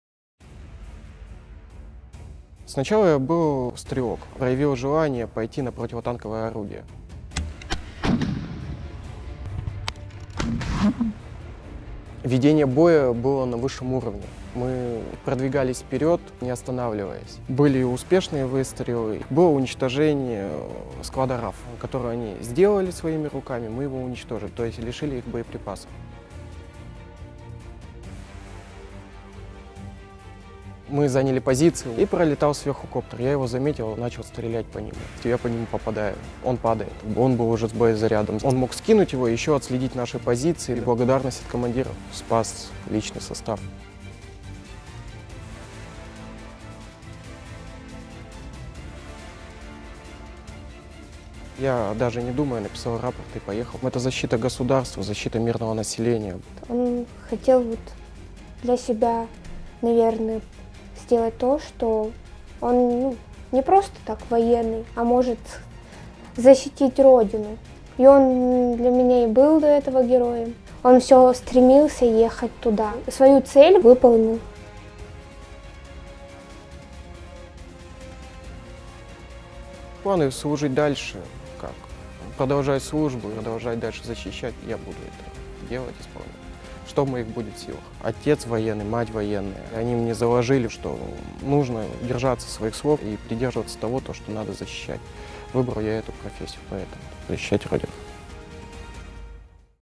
The story of the hero Vladimir Doroshko on the Artemovsky direction.
“The conduct of the battle was at the highest level. We moved forward without stopping. There were successful shots. There was the destruction of the RAV warehouse, ”says the commander of the anti-tank crew.

🎯 Once, thanks to his vigilance, Vladimir was able to save an entire unit from shelling. He shot down a copter with a warhead.

💪 Vladimir Doroshko went to the front as a volunteer. “This is the protection of the state. Protection of the civilian population,” the fighter believes.

💉 In one of the battles, Vladimir was seriously wounded, but he already dreams of returning to the service.